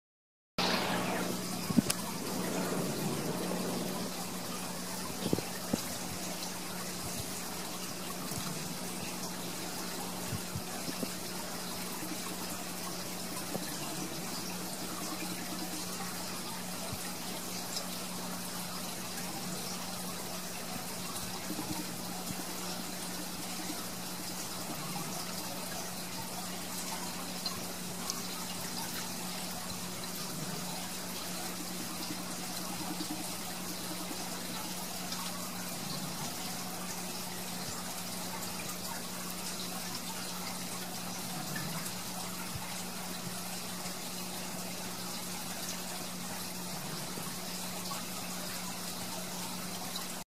HAKE in their new summer home
2 Hake from Long Island Sound in the Marine Education Center's aquarium in Harbor Island Park, Mamaroneck, NY